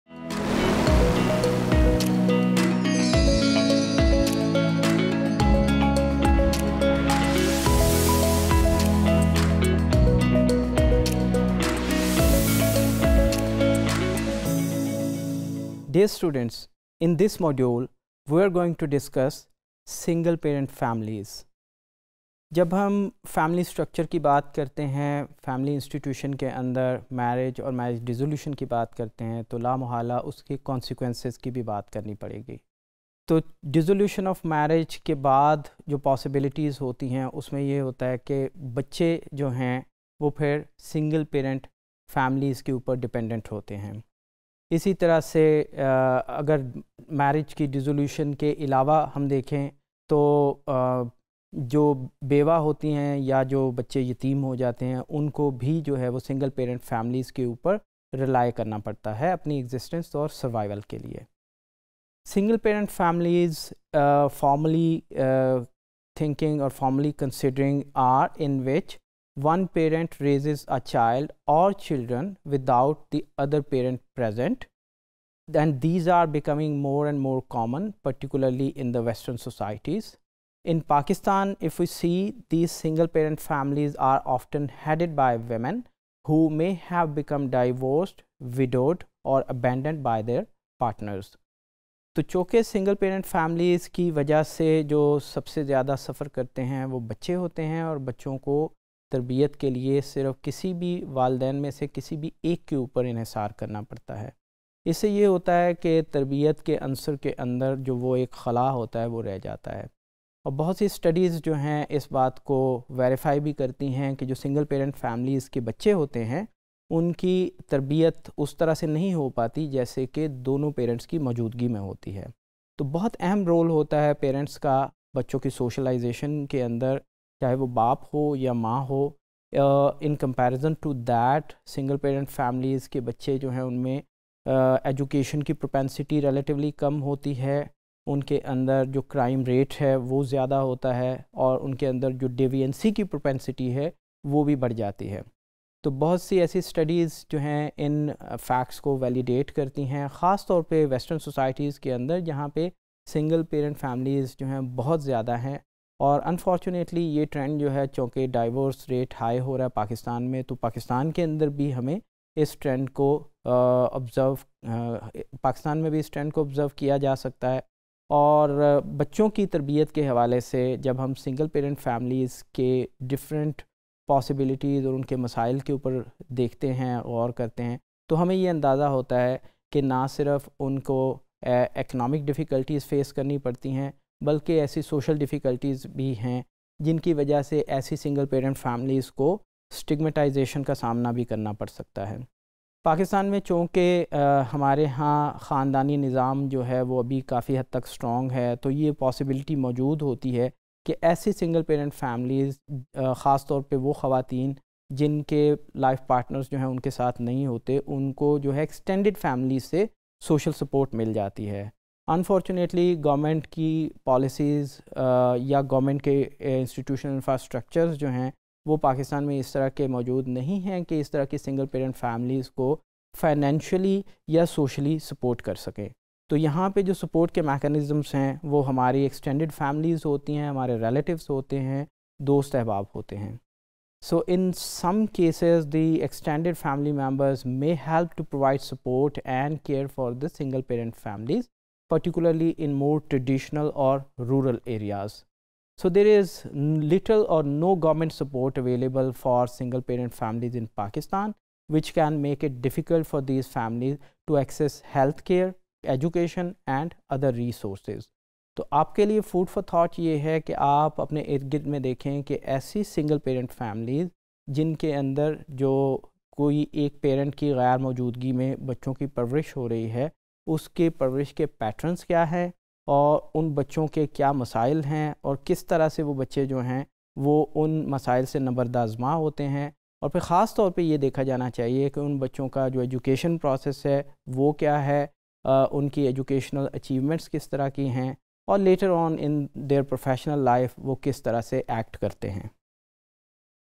Trends in Family: Single Parent Families | Introduction to Sociology | SOC101_Topic143
Topic143: Trends in Family - Single Parent Families,
By Dr. Imran Sabir